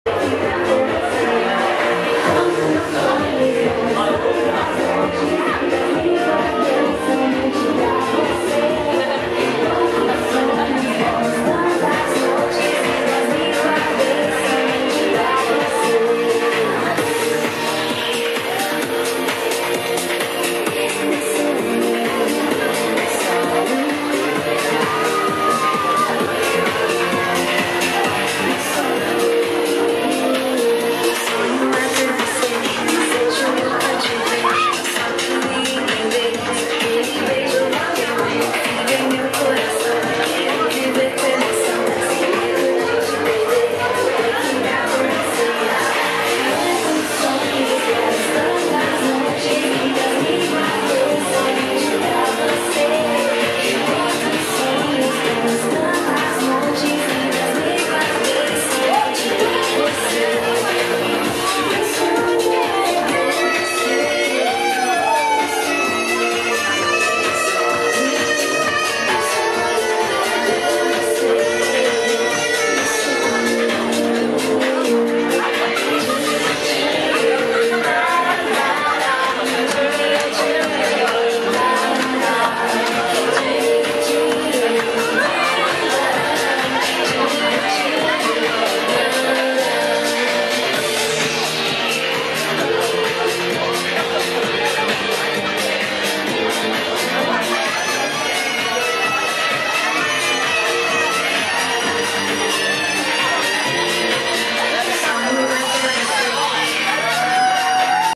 《 ﾒｲﾝﾌﾛｱ一周 》09.07.19 HAPPY SPACE at shibuya NEO
デイタイムの渋谷で楽しめ、クラブ初心者からベテランクラバーまで笑顔で集えると評判のクラブイベントの模様です★